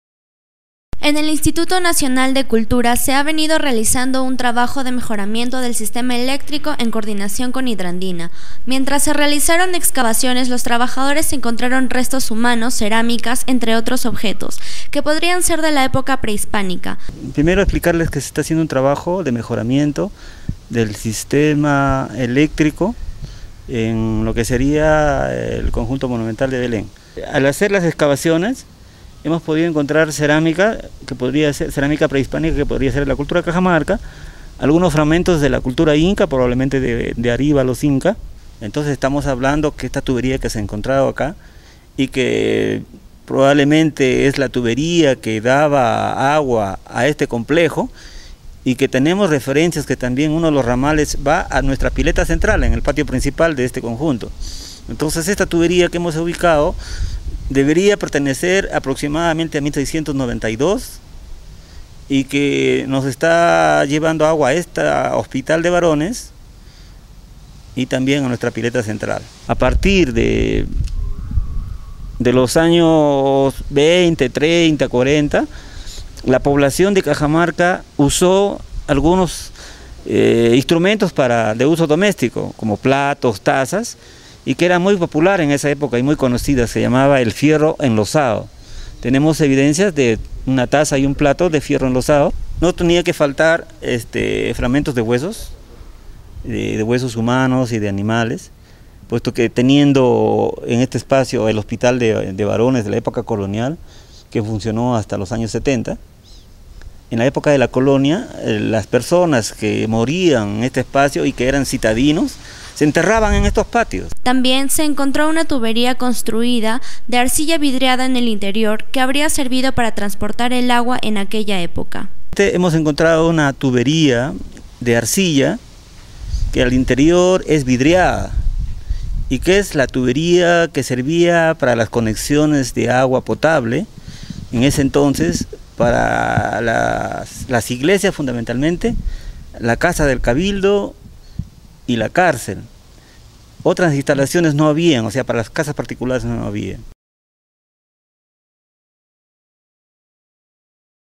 Hallazgos en el complejo Belén
En el instituto nacional de cultura se ha venido realizando un trabajo de mejoramiento del sistema eléctrico en coordinación con hidrandina.
Mientras se realizaron excavaciones los trabajadores encontraron restos humanos, cerámicas entre otros objetos que podrían ser de la época pre-hispánica, también se encontró una tubería construida de arcilla vidriada en el interior que habría servido para transportar el agua en aquella época.